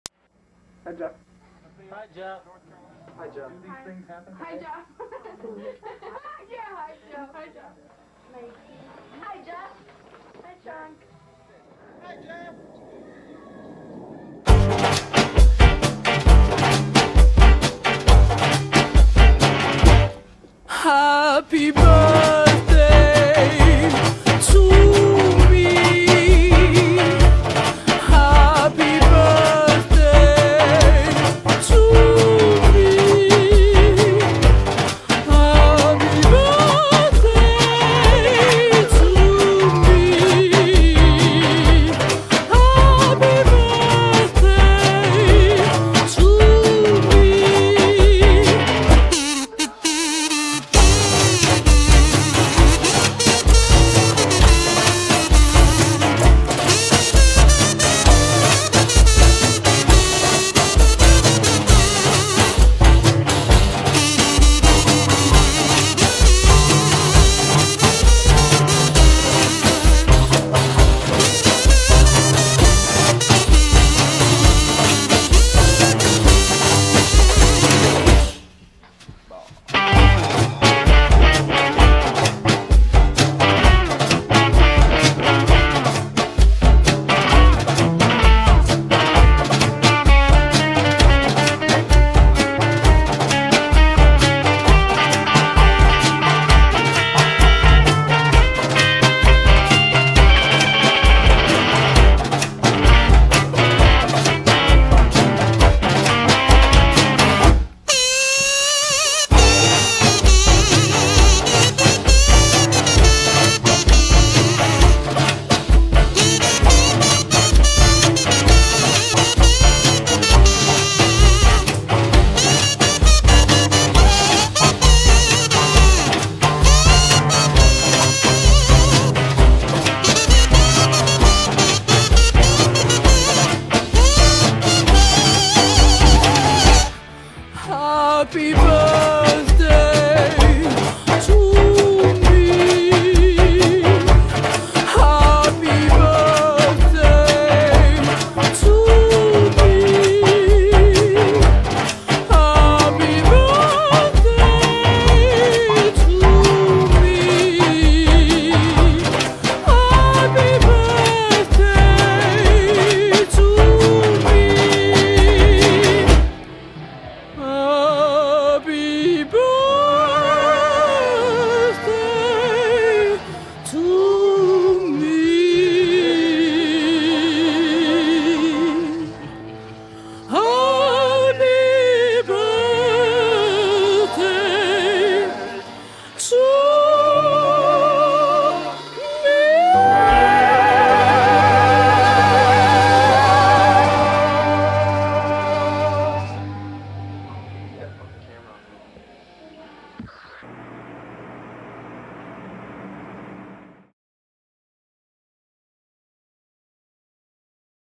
Jeff's Birthday Video
Wishing my ole pal Jeff Schmitt a happy birthday